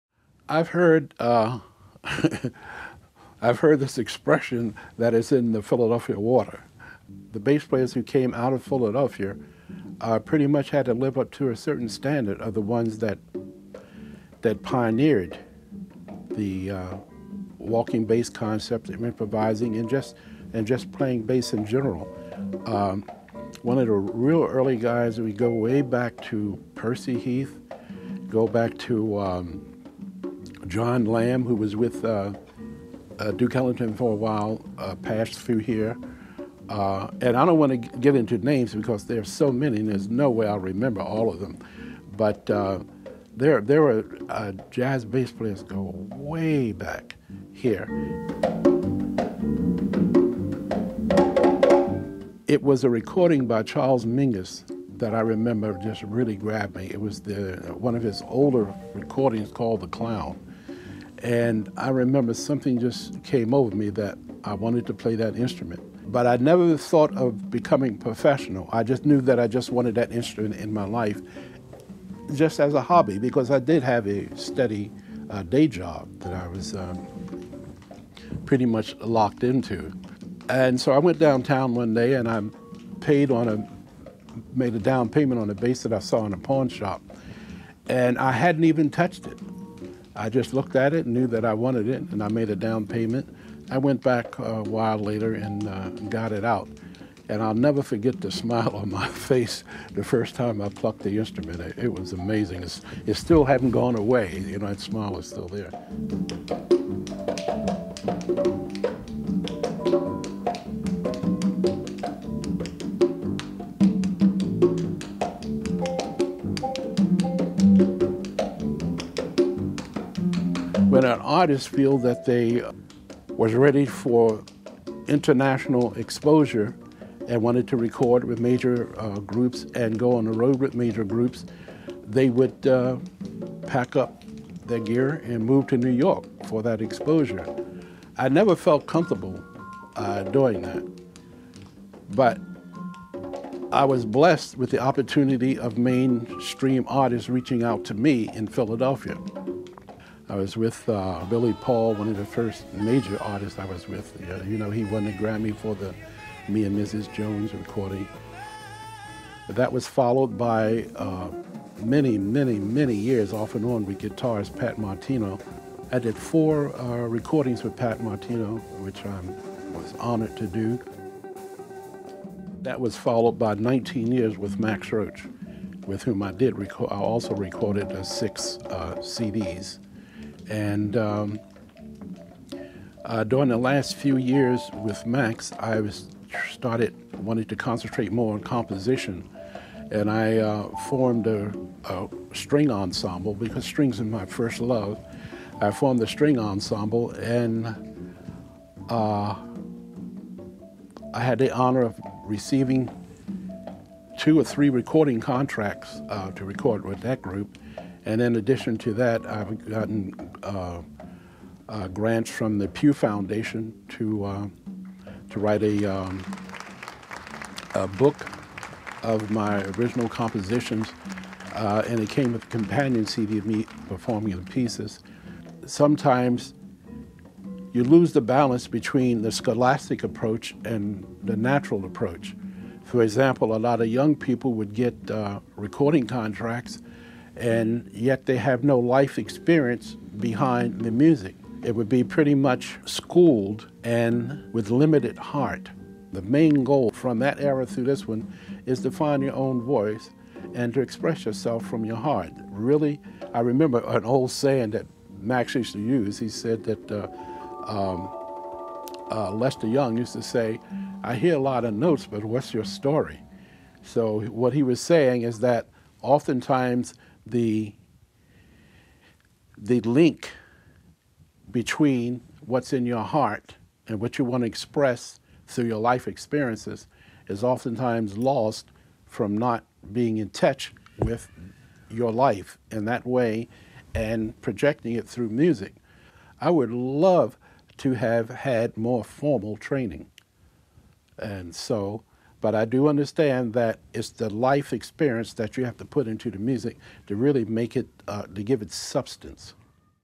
Tyrone Brown
Philadelphia bassist Tyrone Brown breaks it down. With improv performance footage from the Jazz Bridge Collingswood concert series featuring Tyrone with Doc Gibbs, Germaine Ingram and Randy Sutin.